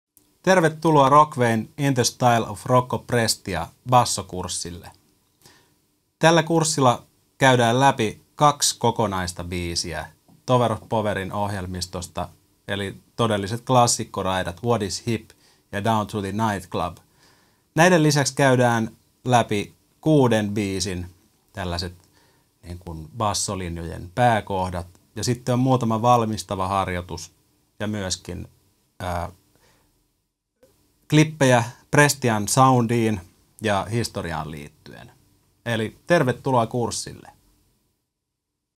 Rocco Prestia (In the Style of -kurssi)
Tulossa 12.10. ja opettajana huikea opettajana Timo Tolonen.
Tiedossa Tower of Powerin riffejä ja kaksi huikeaa kappaletta.